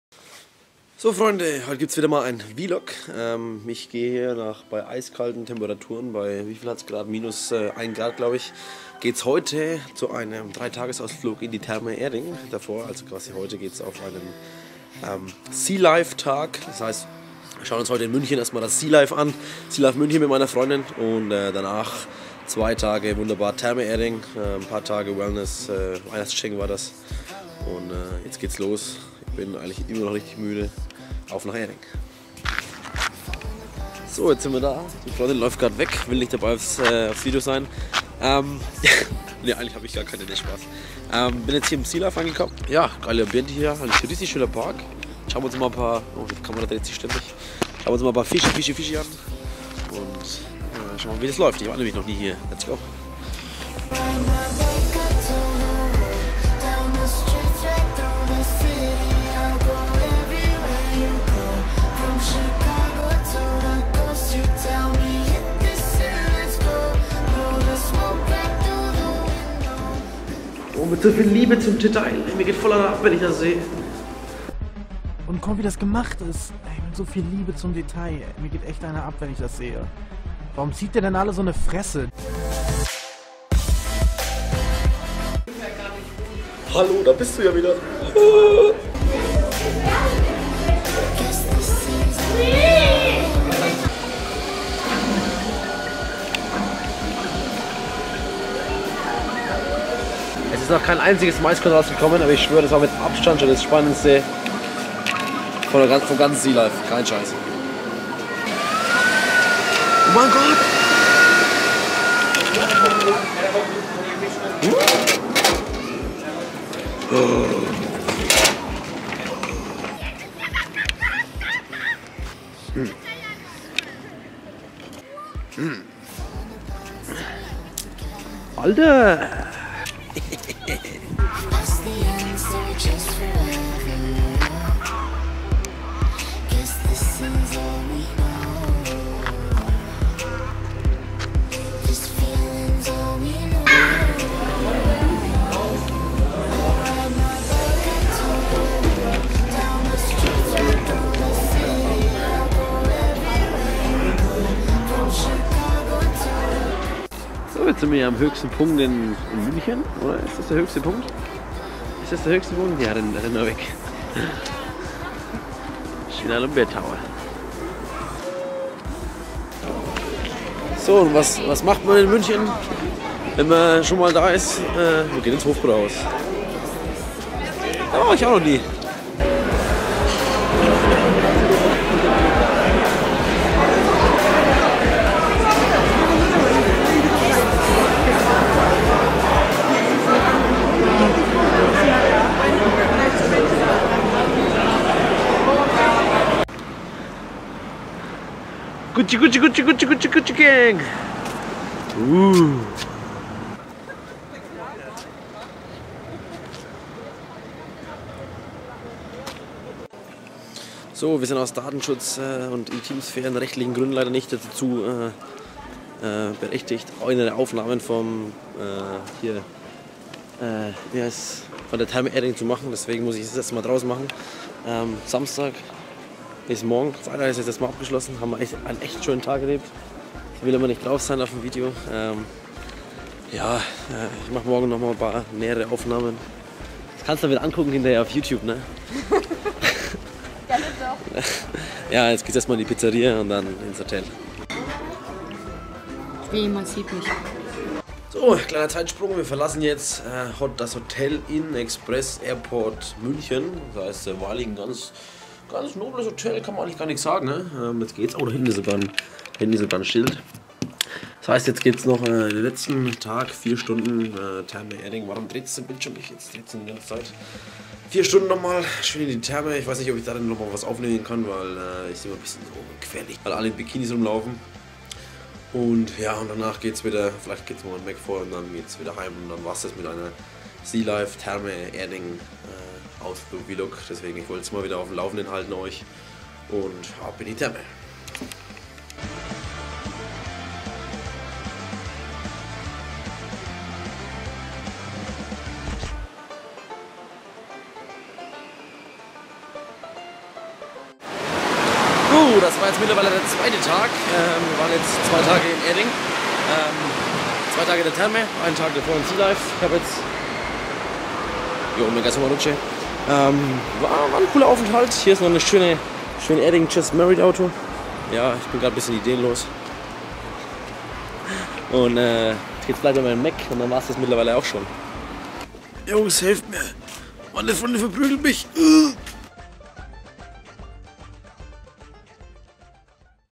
VLOG Therme Erding + Sealife | TwoMillionWays Update
Der TwoMillion trägt Vollbart, spielt ein bisschen weniger Fifa und hat jetzt eine Freundin. Ist viel passiert in letzter Zeit.
Viel Spass auf meinem VLOG zur Therme Erding sowie Sea Life München
 TwoMillionWays